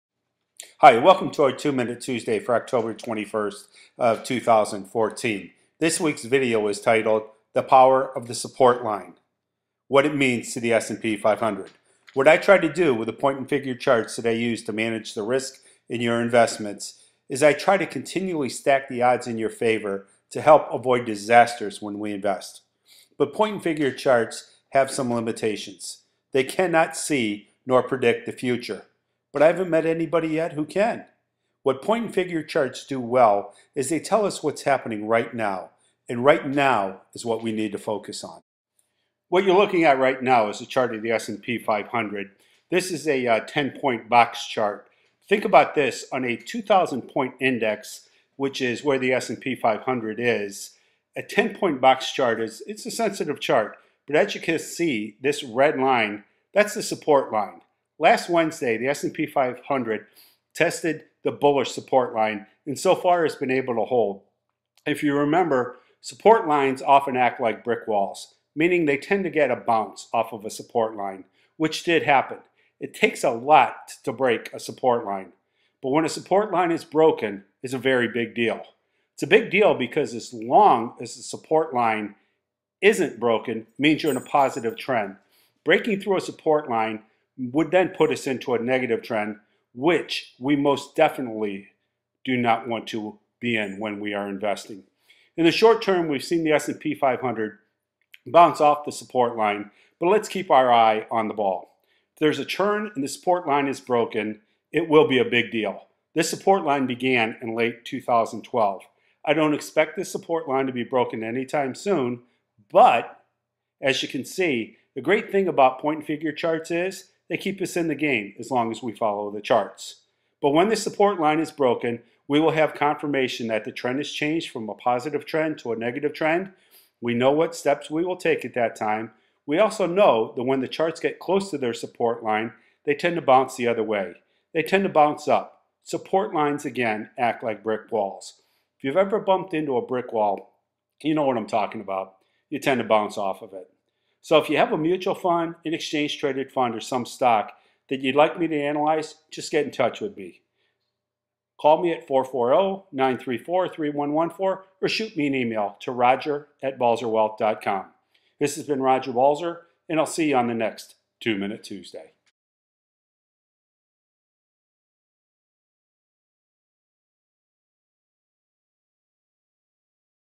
Two-Minute-Tuesday 10-21-14
Roger gives his two cents about the power of support lines.